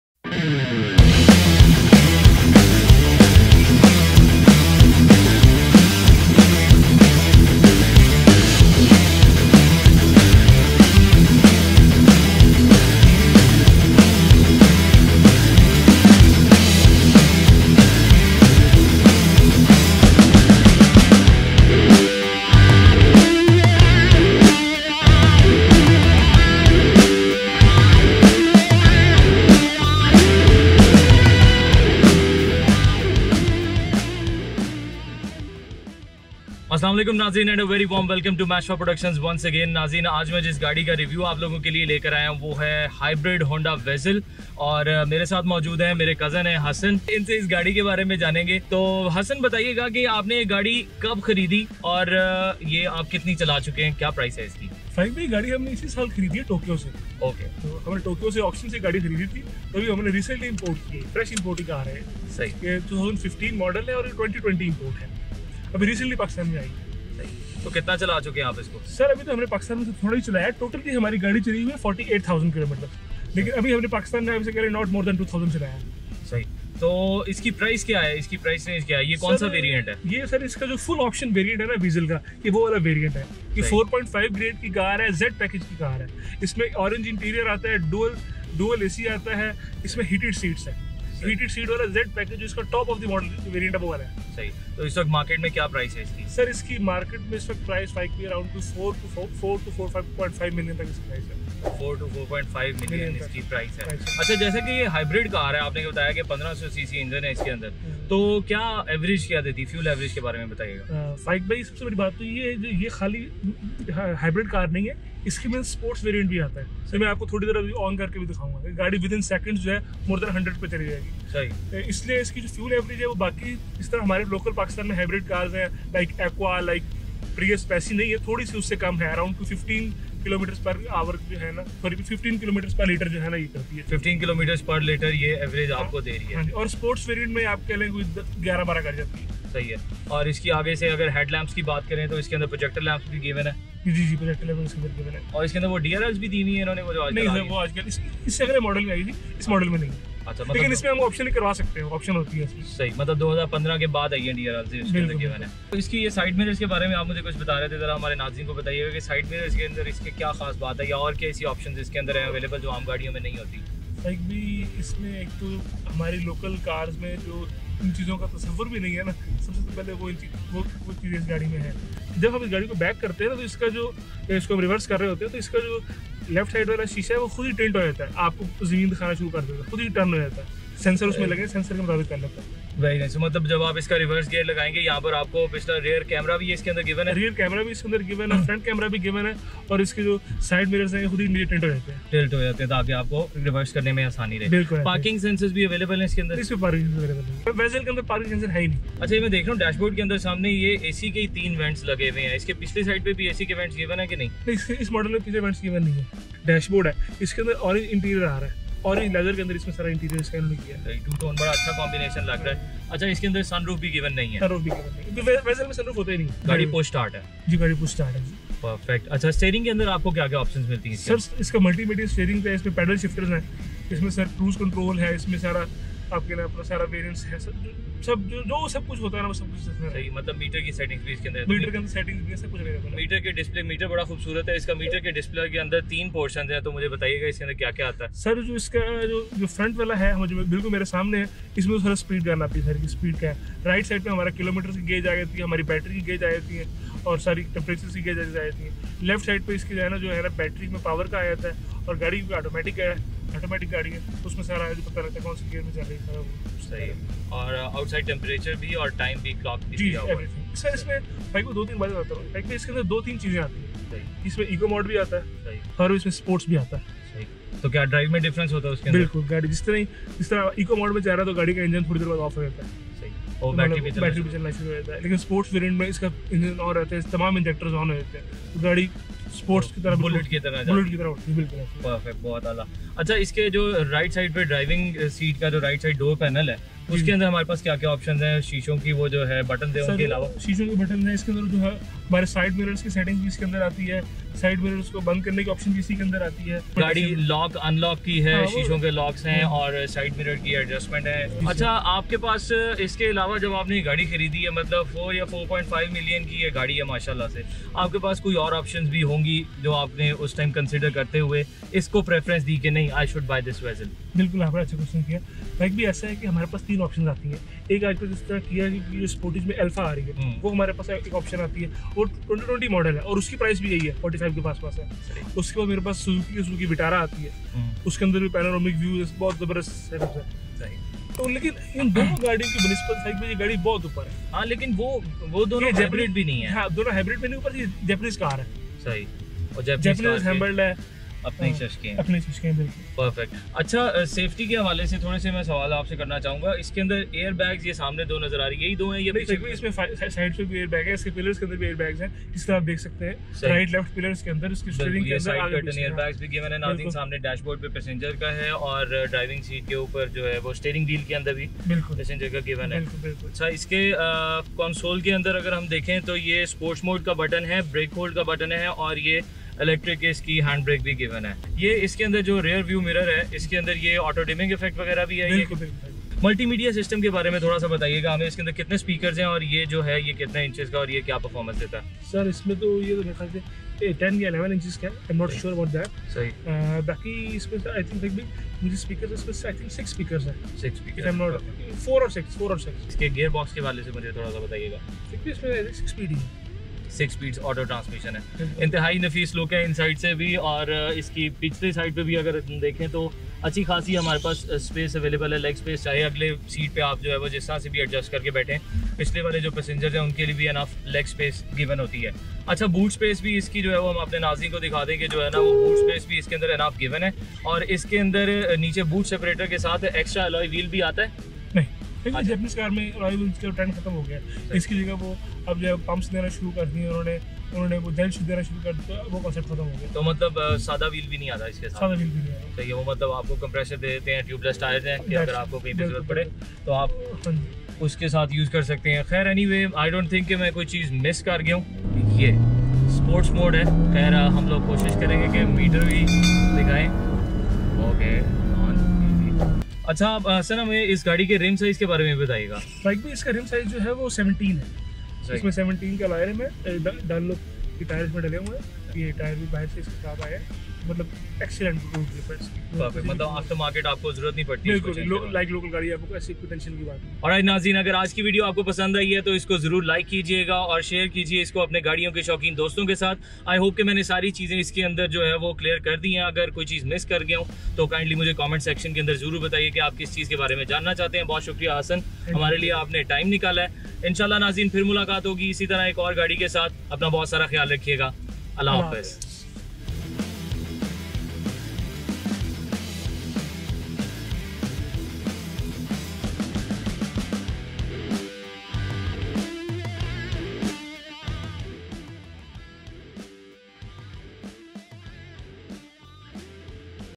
Honda Vezel Hybrid | Owner's Review | 2020
Have a look at brand new Honda Vezel 2020 in Pakistan. In this video you will get owner's review about the new features and performance of the variant imported from Japan. If you are planning to buy or just want to know about features of Honda Vezel 2020 this video is for you.